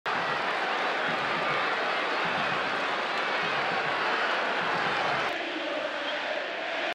Gol, amarilla y roja para Franco Jara en el buen triunfo de Belgrano vs Banfield
26/06/2023
Liga Profesional de Fútbol de Argentina 2023